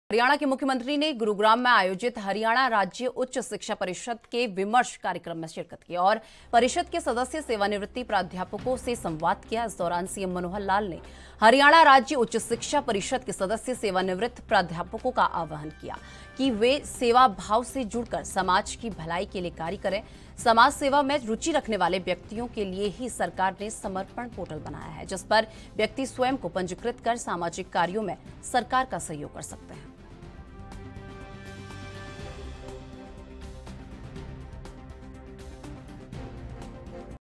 गुरुग्राम में ‘विमर्श’ कार्यक्रम का आयोजन, मुख्यमंत्री मनोहर लाल ने की शिरकत | ANB NEWS
गुरुग्राम में ‘विमर्श’ कार्यक्रम का आयोजन
मुख्यमंत्री मनोहर लाल ने की शिरकत
सेवानिवृत्त प्राध्यापकों से किया आह्वान
समाज की भलाई के लिए करें कार्य- सीएम